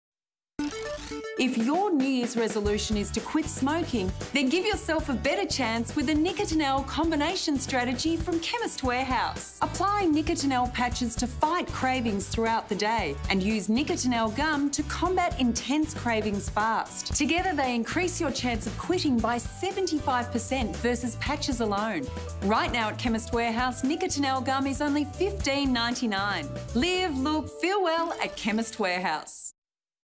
Healthy Break Nicotinell 96 pack 2mg Mint & Fruit Pack
Created for Quattro Advertising. Part of a series of TVCs for Chemist Warehouse, featuring Jo Silvagni. Producer Deb Hilton-Silver, Director Harvey Silver, First Assistant Director Brent Houghton, DOP Warwick Field. Post at Method Studios.